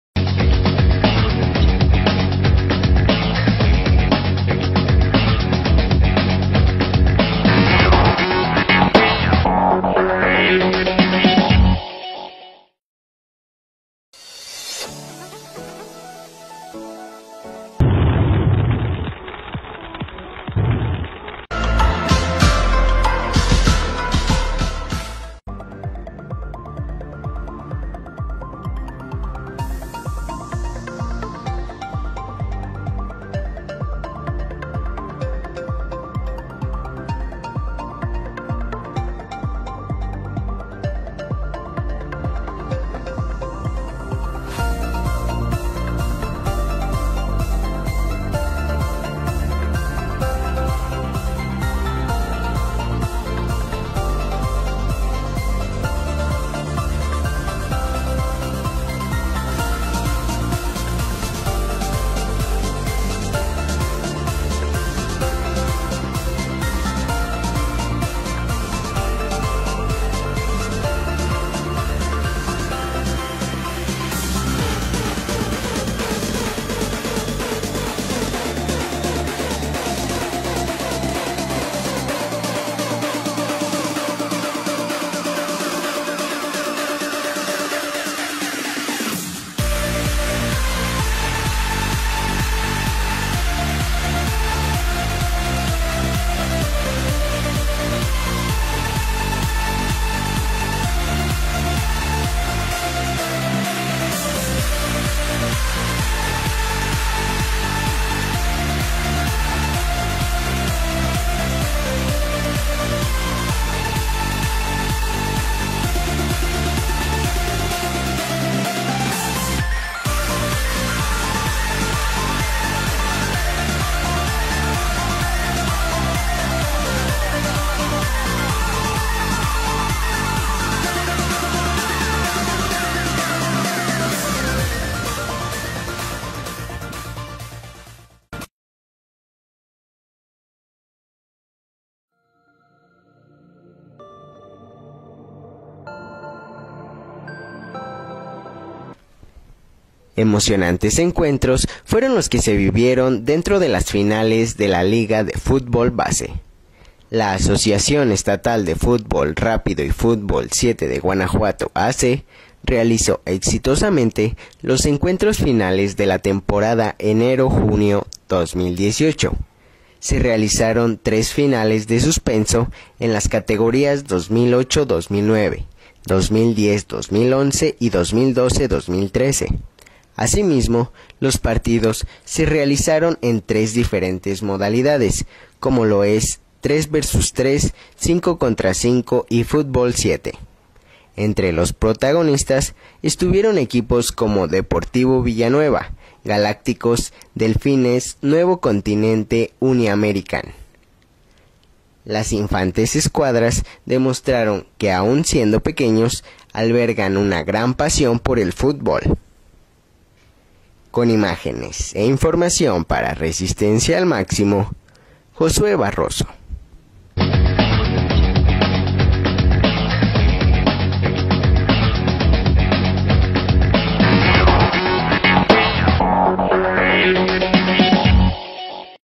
FINALES DENTRO DE LA LIGA BASE con Juegos de FUNiño en México
EQUIPOS COMO DEPORTIVO VILLANUEVA , Galacticos Celaya, Uniamerican International School Y DELFINES DE Nuevo Continente Celaya, FUERON LOS PROTAGONISTAS DE LAS FINALES DENTRO DE LA LIGA BASE TEMPORADA ENERO-JUNIO 2018 con Juegos de Fútbol a la medida del Niño FUNiño ( como plato principal) de Competencia.